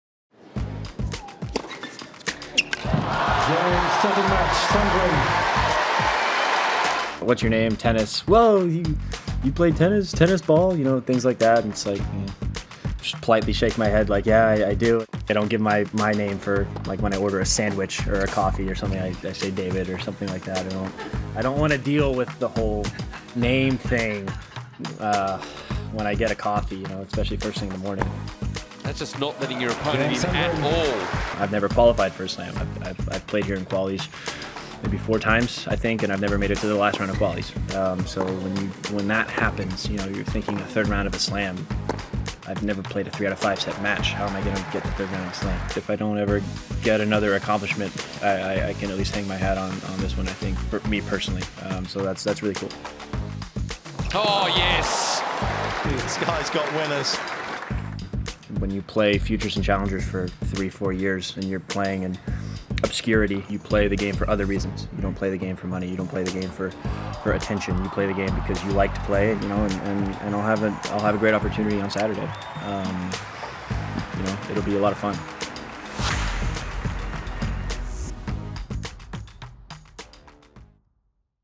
Anyone for Tennys? | Australian Open 2018
Tennys Sandgren talks about his experience at the Australian Open 2018, and the fun-loving nature of Australians about his name.